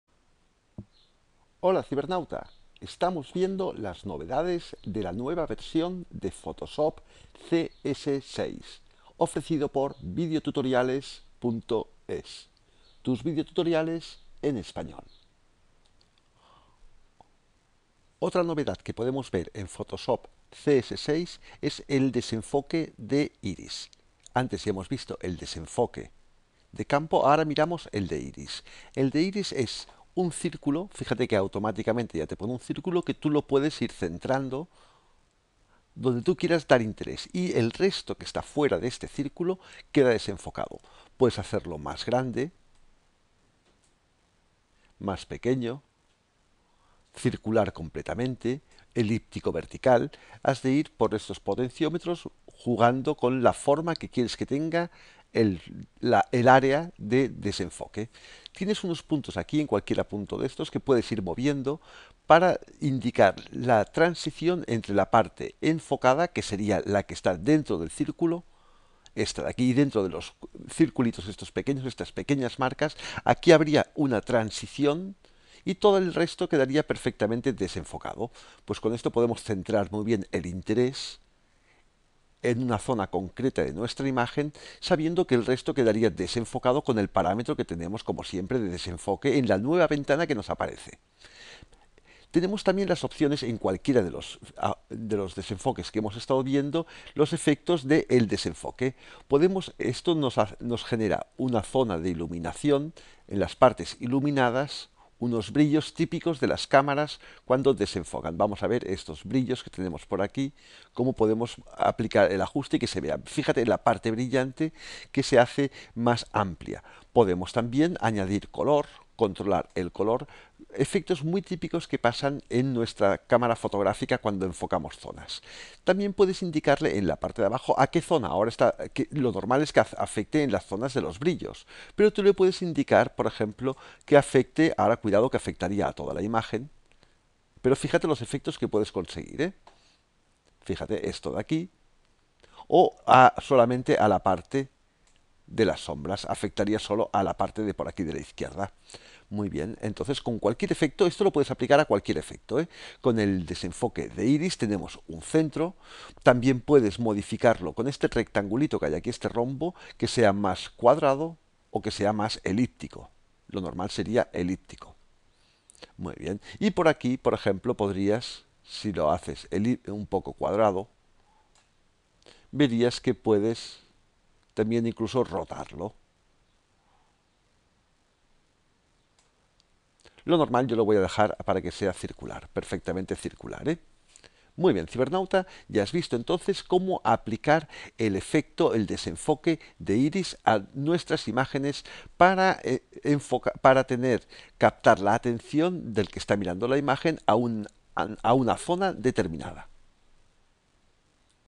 novedades de photoshop cs6 - desenfoque de iris
nos ofrece este nuevo videotutorial en el que nos muestra como aplicar el desenfoque de iris a una imagen para crear interes en un área concreta. Puedes ver todas las novedades en la web